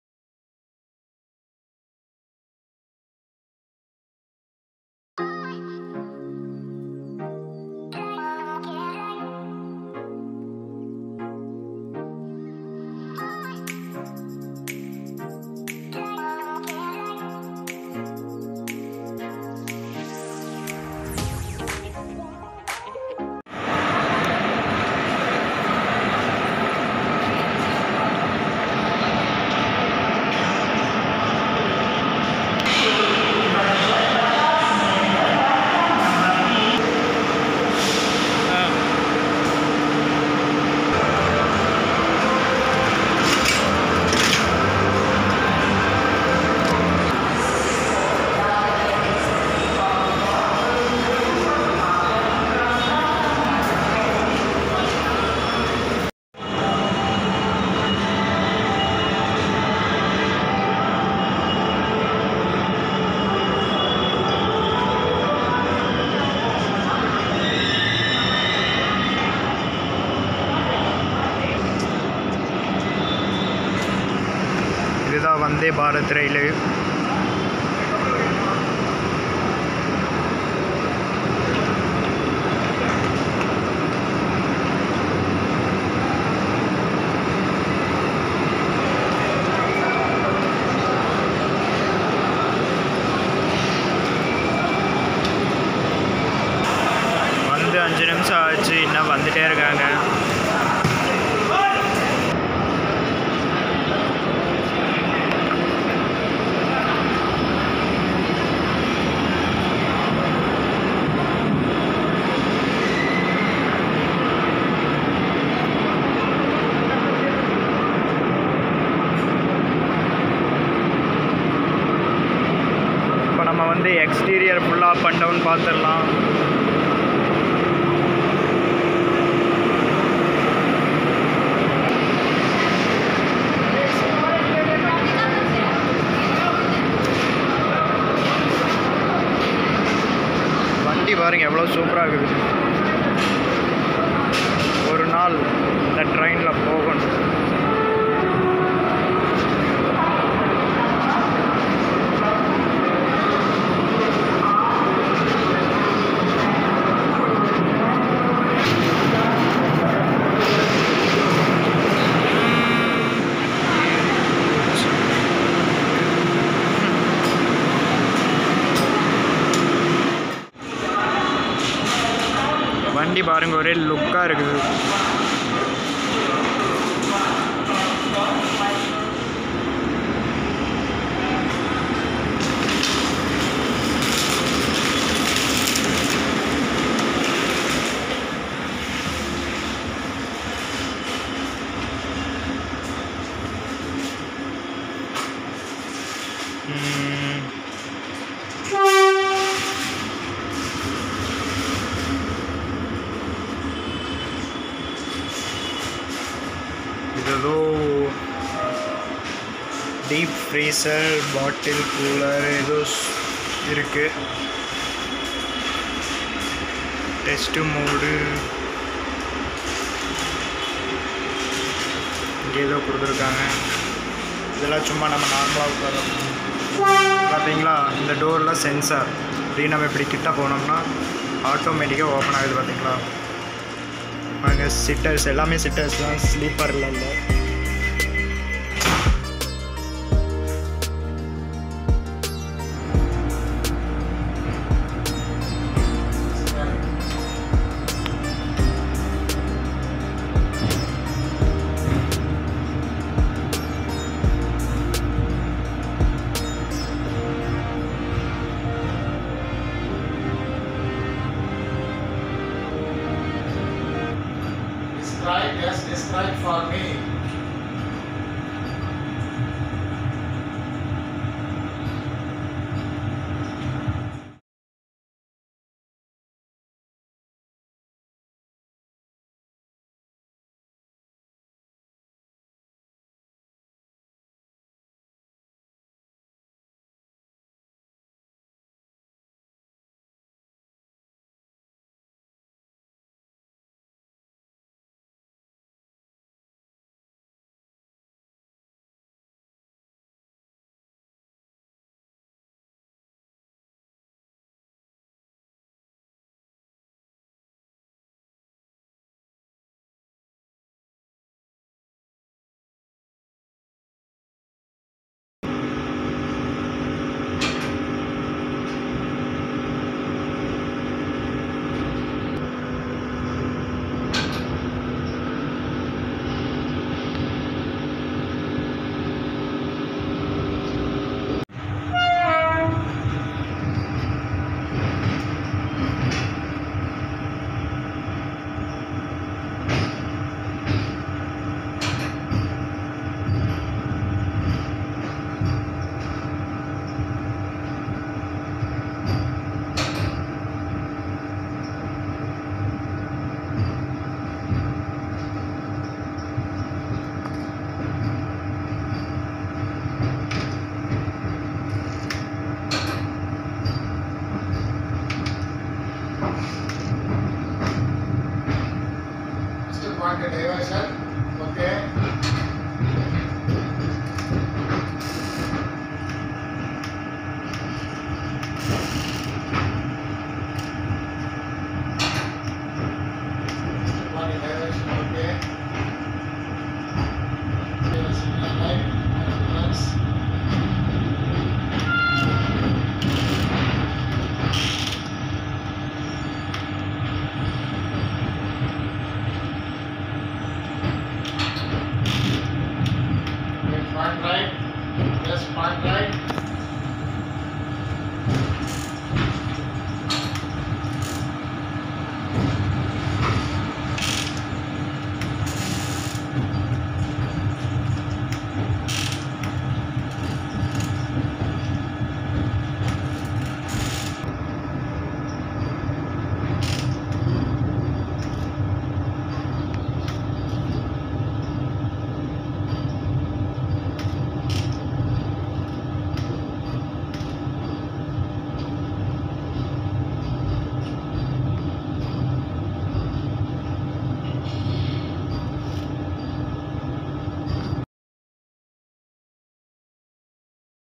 Chennai 🔁 Mysore Vande Bharat Train
You guys can see Chennai to Mysore Vande Bharat train, which came to Chennai central railway station. Thambu showcased interior and exterior of Vande Bharat Train and also loco pilot cabin as well.